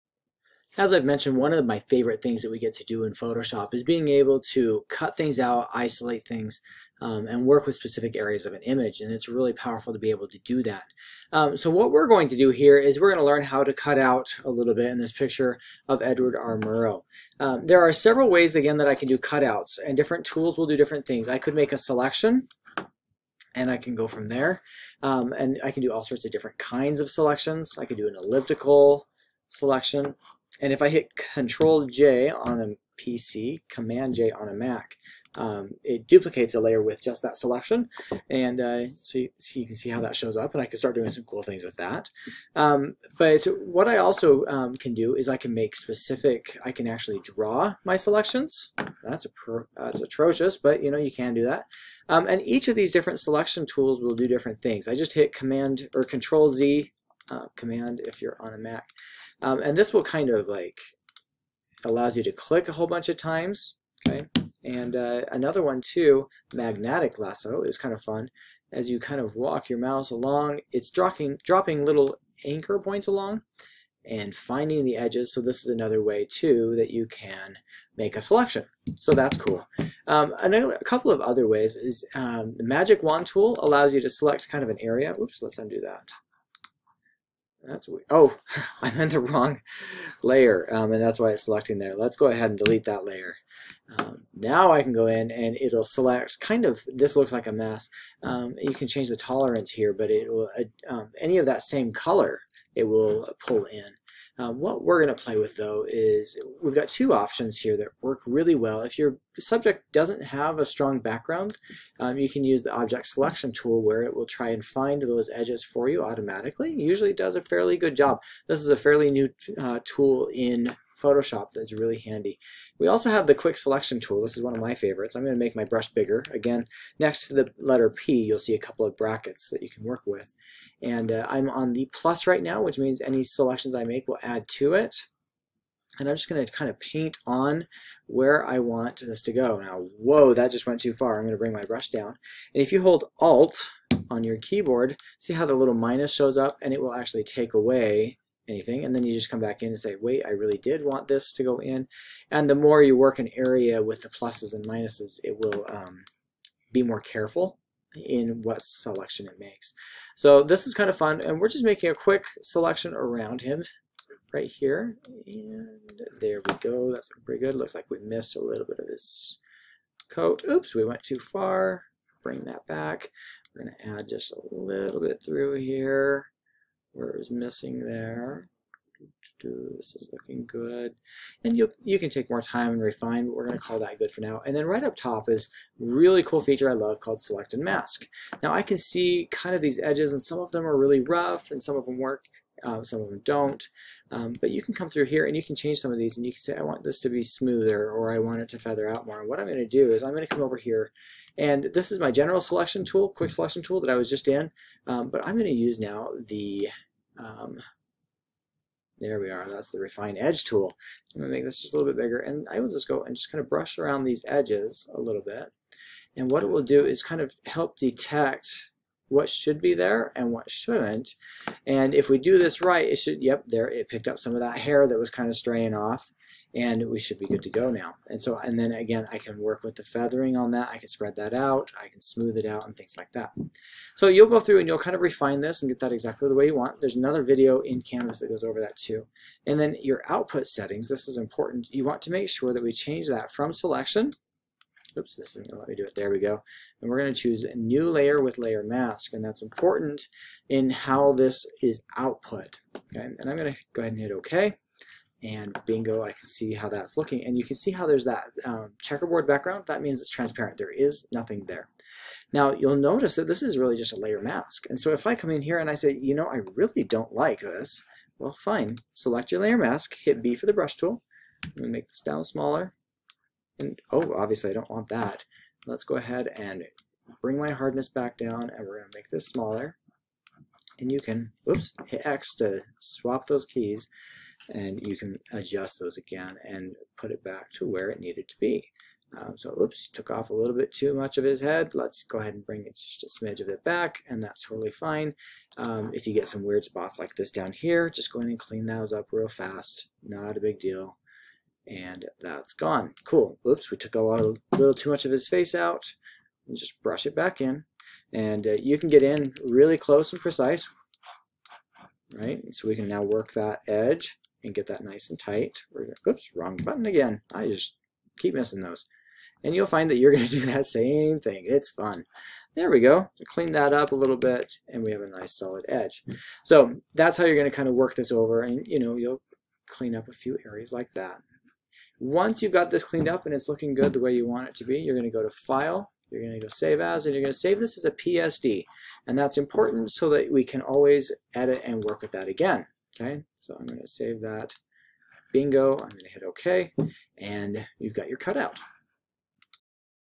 Cutting out & refine edge
Time to learn how to cut out an image!